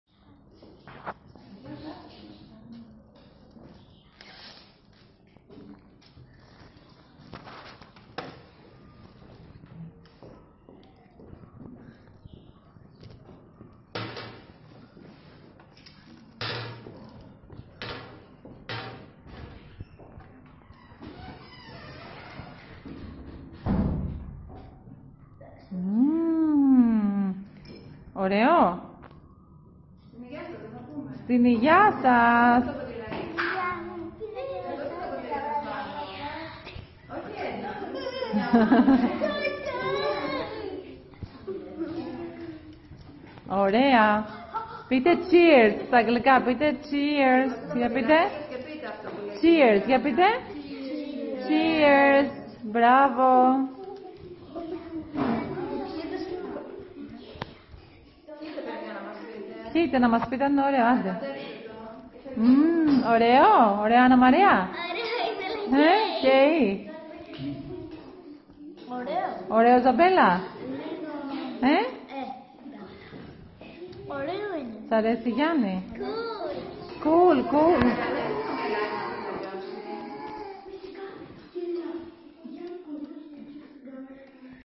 making tea with herbs -4th grade -8th primary school chios
etwinning program 2018-19-use less water.